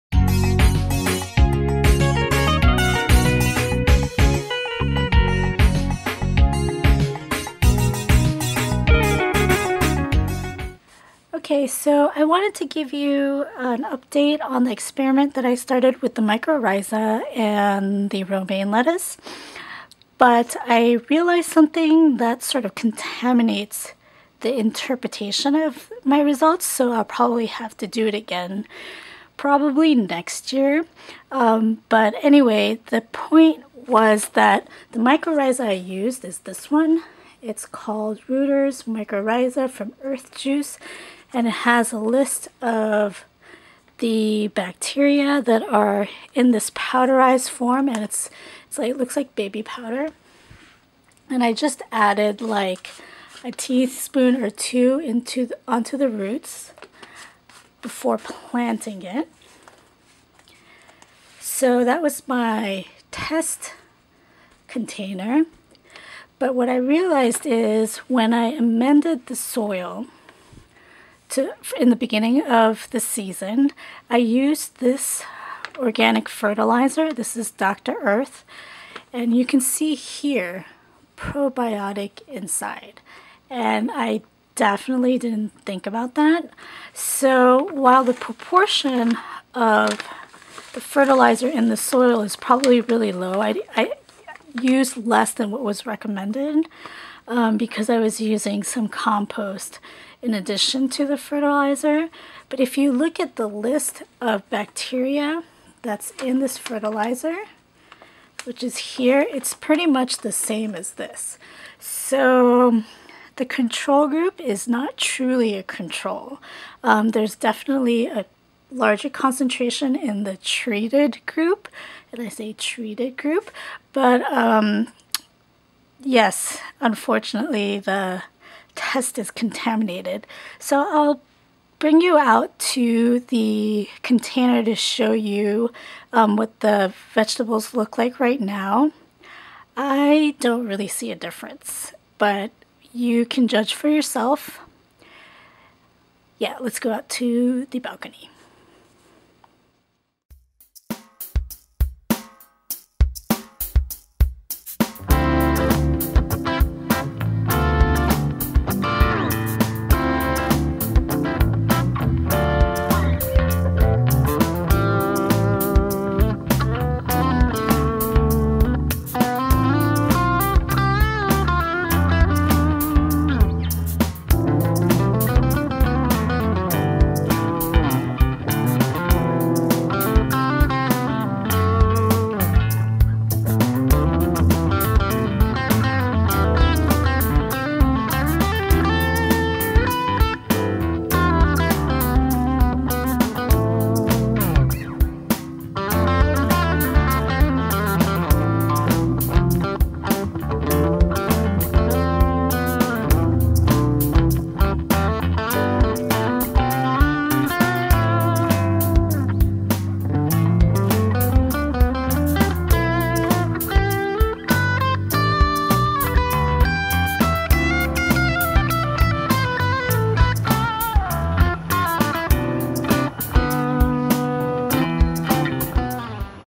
Mycorrhiza experiment update - romaine lettuce and bok choy
This is an update to my mycorrhiza experiment. I noticed that I made a little...well, kinds of a big mistake but I show the results anyway. The link to my first video of the experiment is

A little disappointing but just try and try again. At least I have two containers of beautiful lettuce and bok choy.